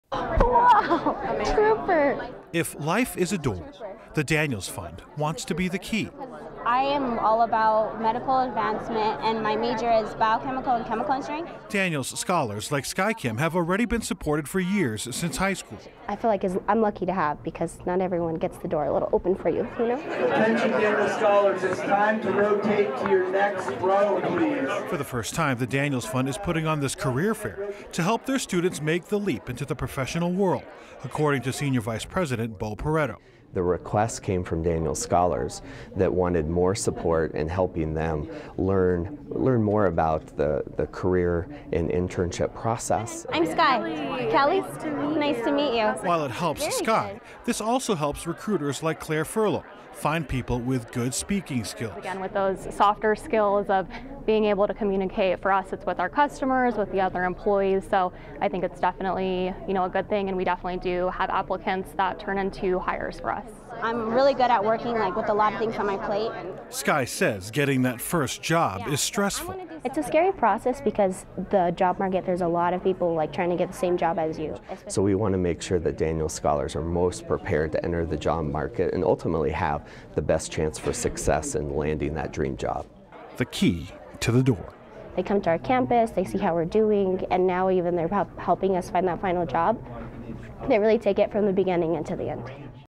Daniels Fund launches SHIFTCareer for Scholars
Named after Bill Daniels, the nonprofit launched something new called "SHIFTCareer 2018." SHIFTCareer is a series of workshops on subjects like resume writing and salary negotiation before a career fair with representatives from 22 companies from the Rocky Mountain region.

9NEWS (KUSA) is located in Denver, Colorado.